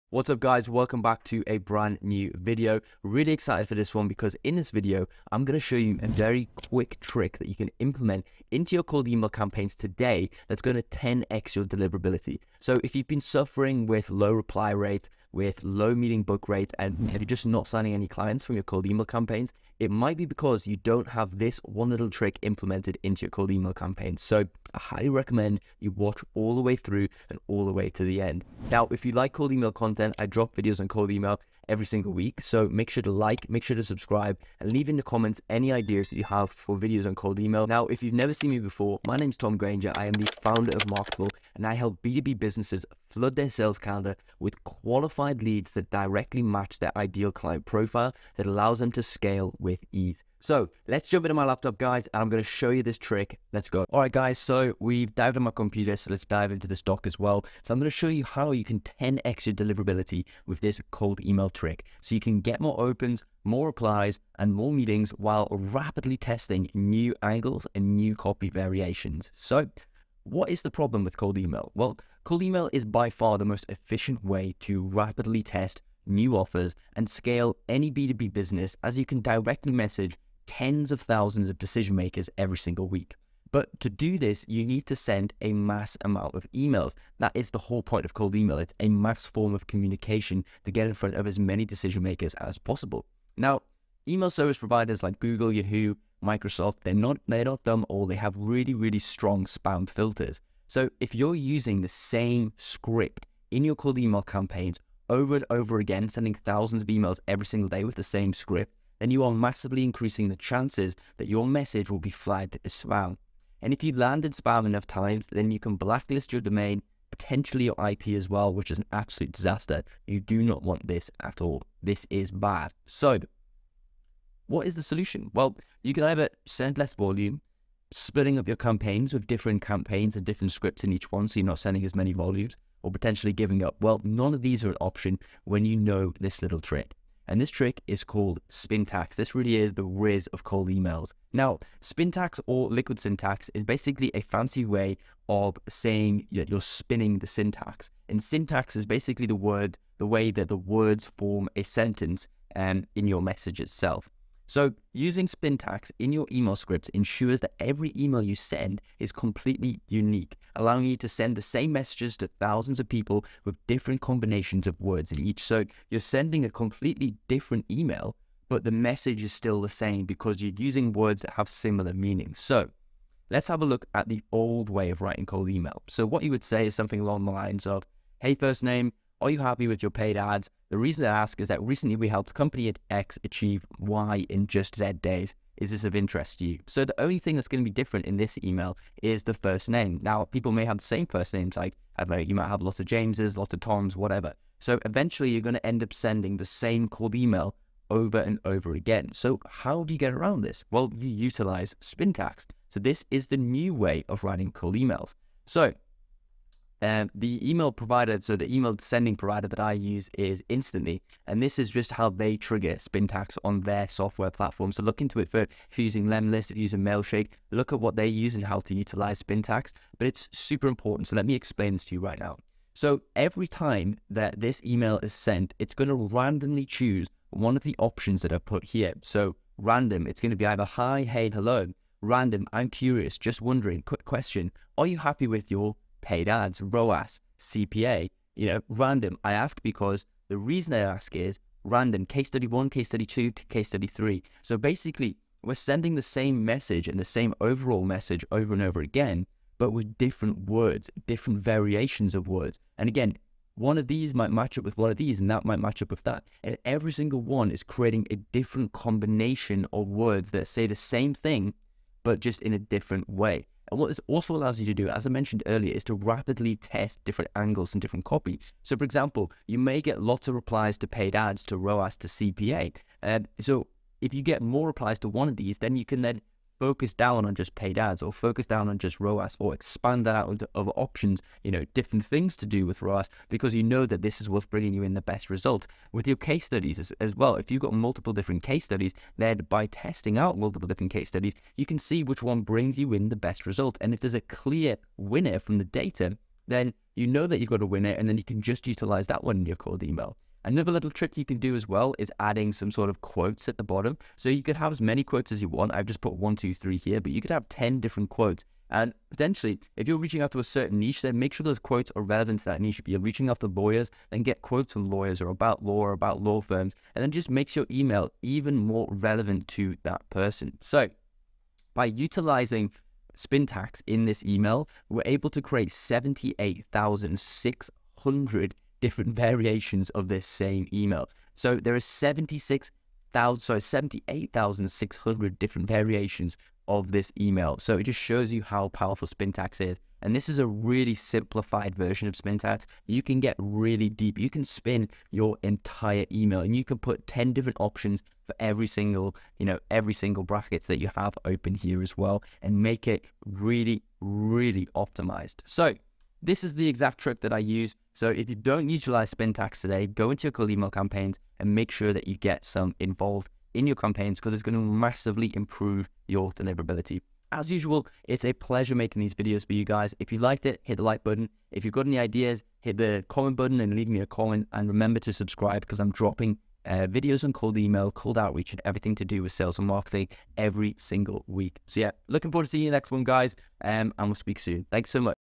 10x Your Deliverability With These Cold Email Tricks
Yo team in this video I'm going to show you a little-known email trick that I use to rapidly test copy variations on cold audiences while improving my deliverability. So if you've been dealing with poor cold email deliverability, this video is for you.

Chapters:
00:00:00 - Increase Deliverability with this Cold Email Trick
00:02:23 - The Importance of Spin Tax in Cold Emails
00:04:51 - The Power of Spintax for Email Optimization